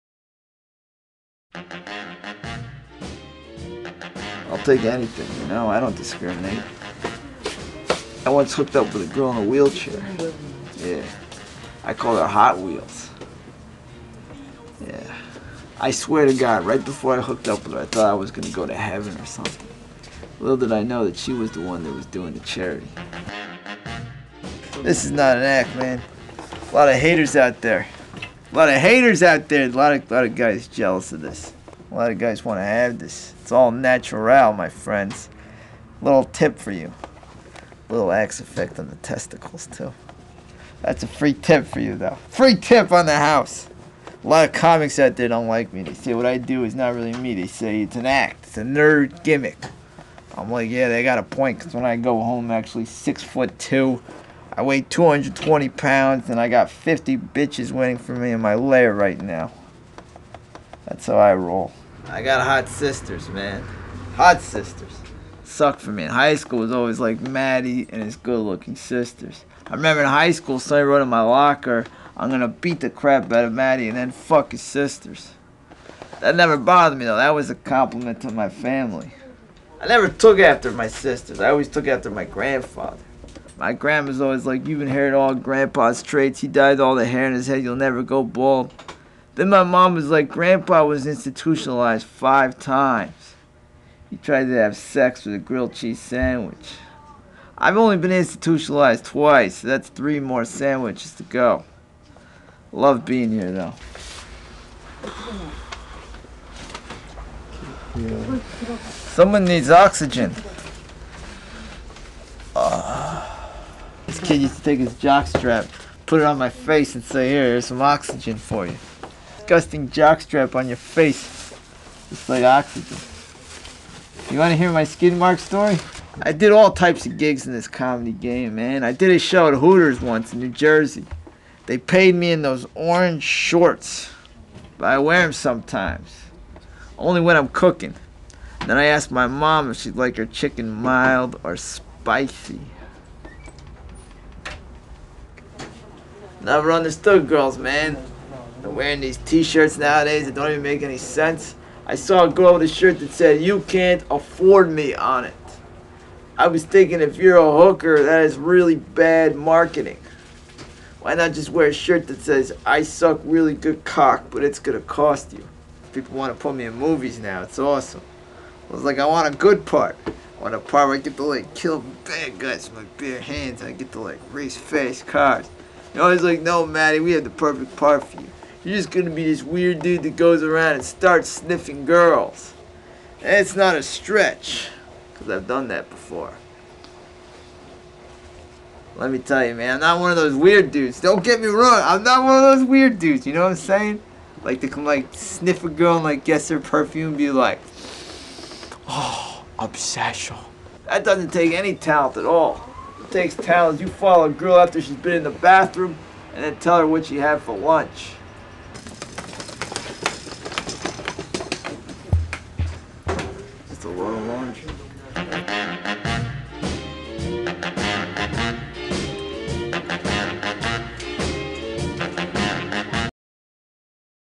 Matty Goldberg Comedy2go
Matty Goldberg sniffs his way to hilarity at a laundromat